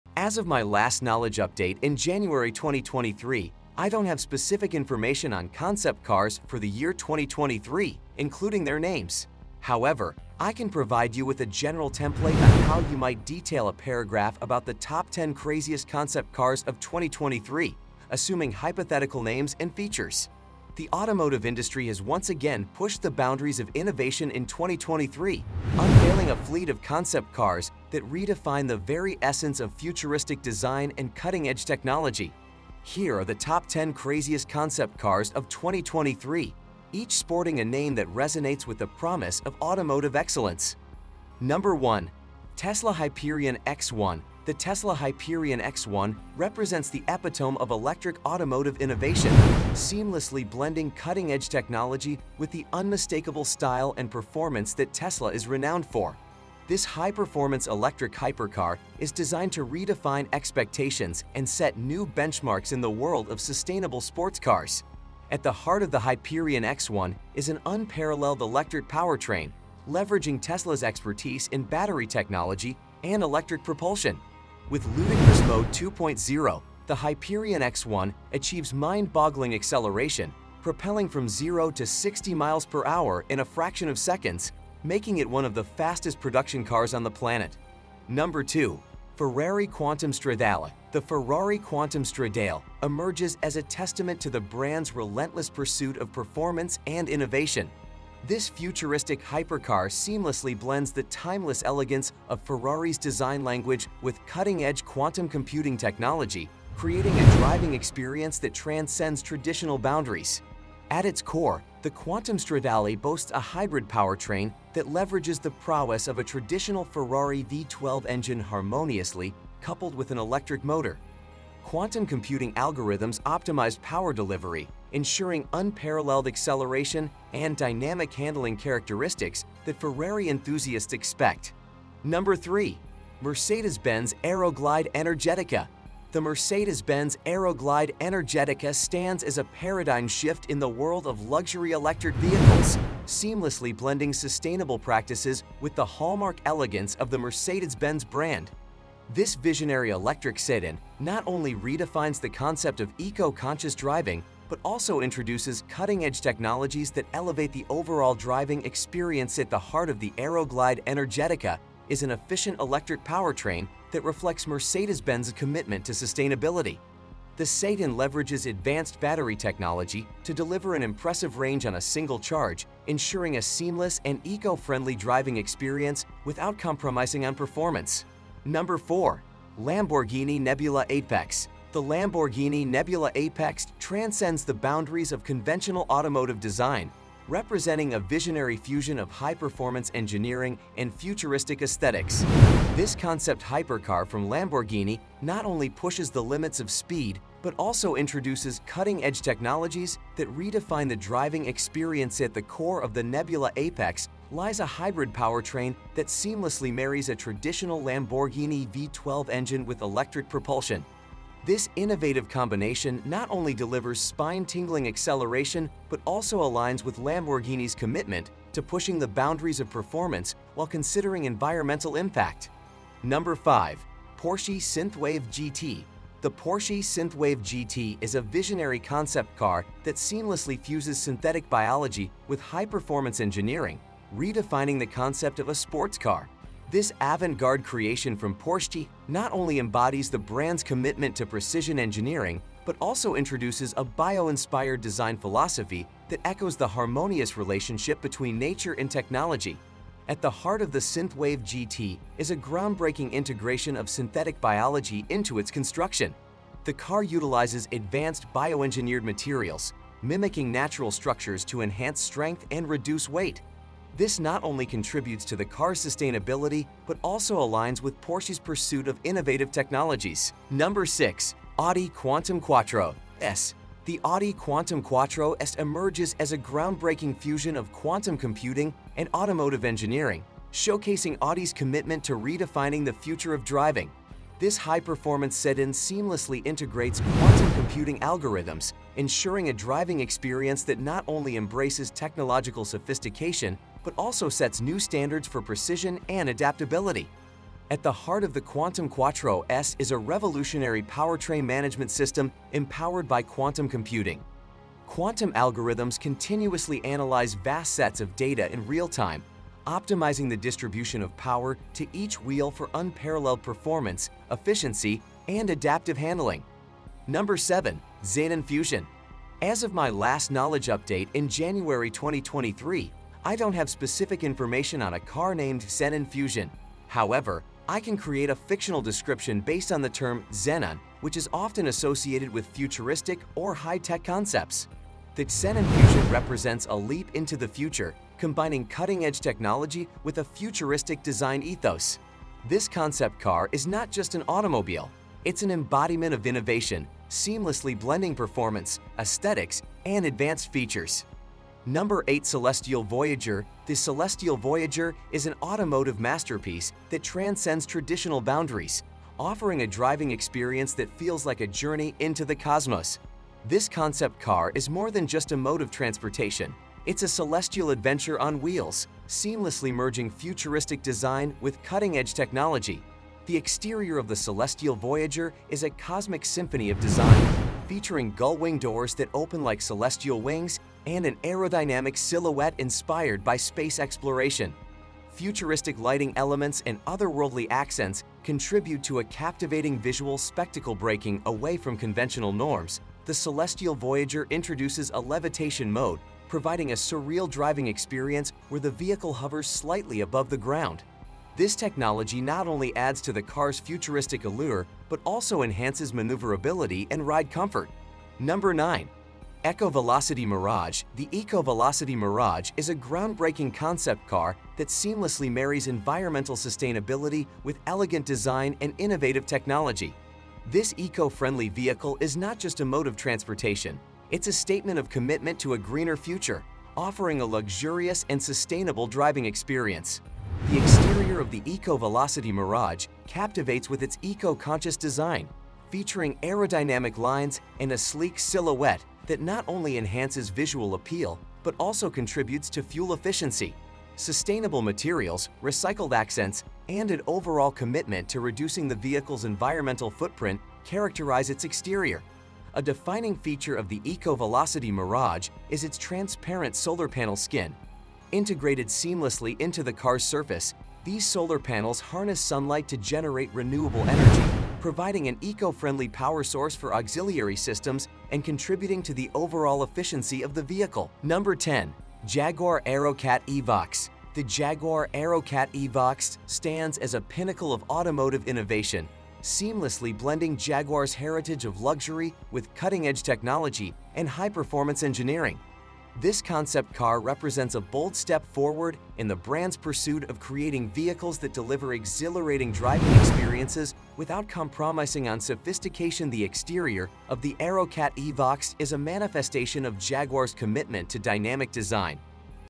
Top 10 Craziest Concept Cars of 2023
Title: "Unveiling the Future: Top 10 Craziest Concept Cars of 2023"

Description: Get ready to embark on a journey into the automotive future as we showcase the most mind-blowing and innovative concept cars of 2023! In this thrilling countdown, we explore cutting-edge designs, revolutionary technologies, and out-of-this-world features that push the boundaries of imagination. From sleek electric vehicles to futuristic hypercars, these concept cars are a testament to the creativity and innovation driving the automotive industry forward. Join us as we delve into the world of automotive dreams and discover the incredible visions that may soon hit the roads. Buckle up for a ride into the future with our Top 10 Craziest Concept Cars of 2023! 🚗✨